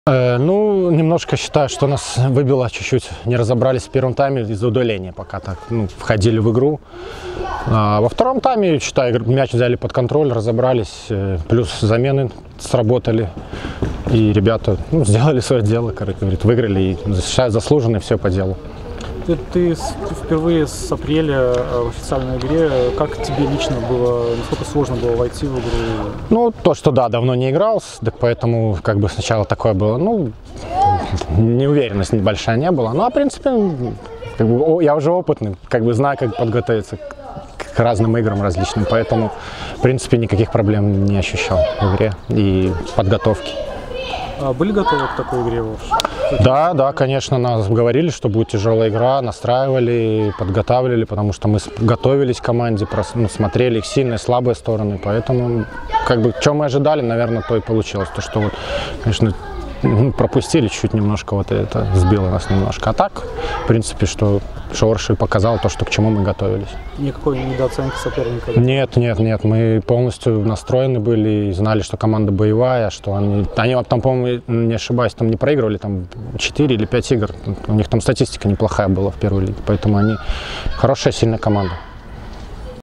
Комментарий Олега Ковалева после матча с «Оршей»
Комментарий голкипера «зелено-белых» после матча 1/16 финала Кубка